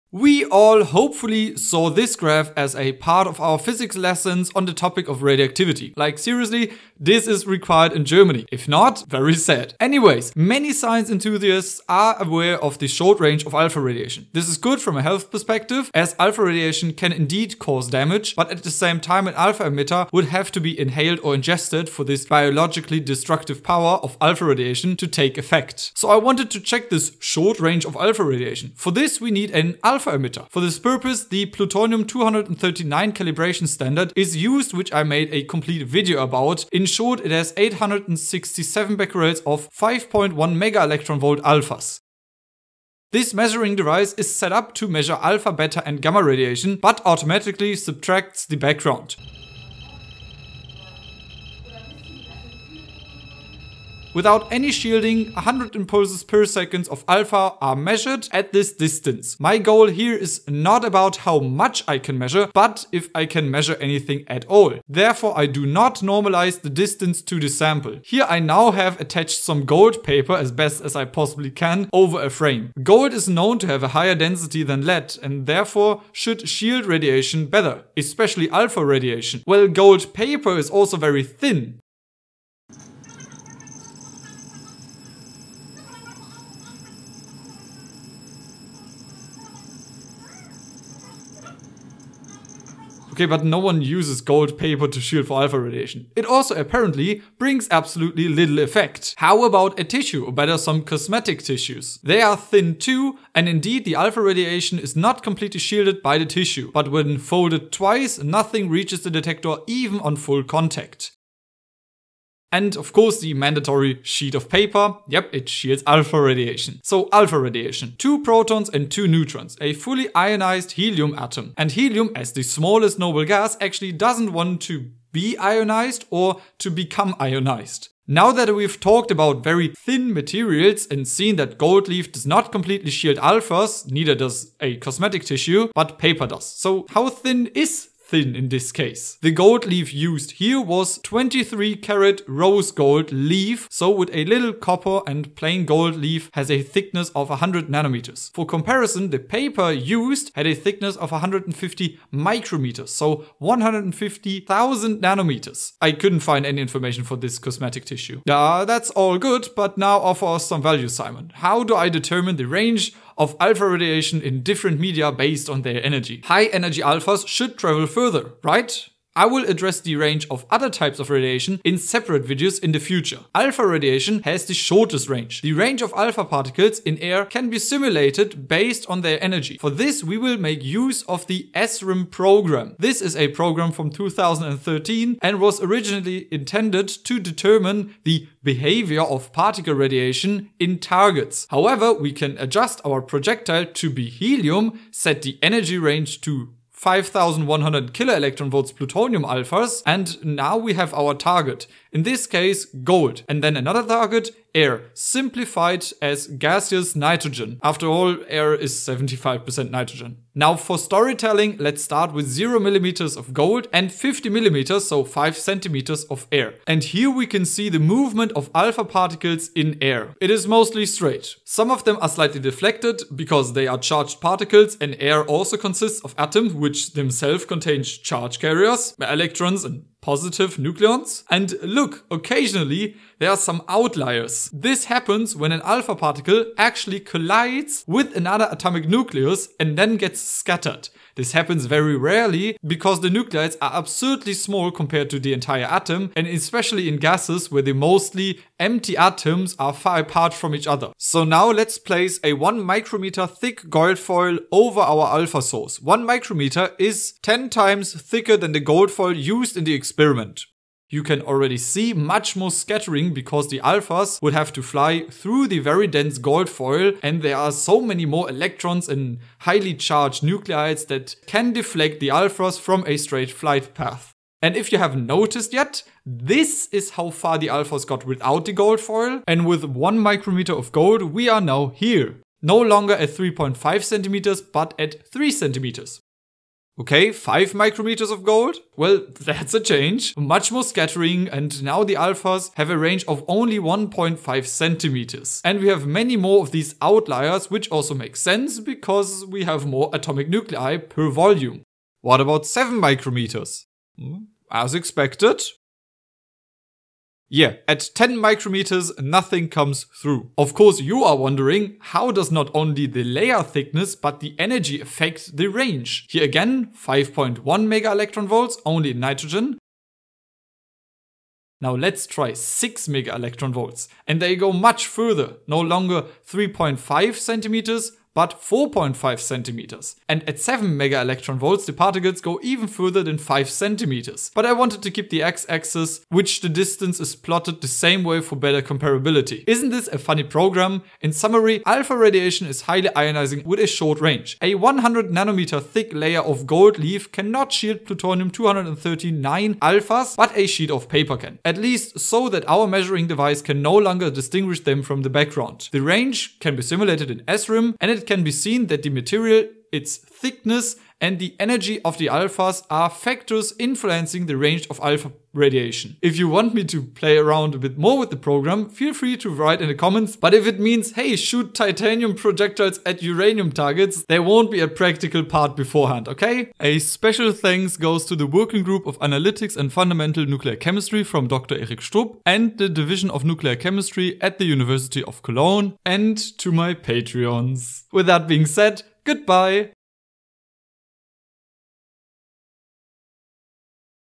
Alpha Radiation - nuclear chemistry
Alpha radiation

In the video, I wanted to demonstrate to you the range of alpha radiation. The experiment with the gold leaf might remind some of you of the famous Rutherford experiment :)

0:00 Intro
0:30 Plutonium Alphas
2:03 Alpha Radiation 101
2:16 Layer Thickness
2:55 Simulation of Alpha behavior
5:39 Summary
6:16 Bye :)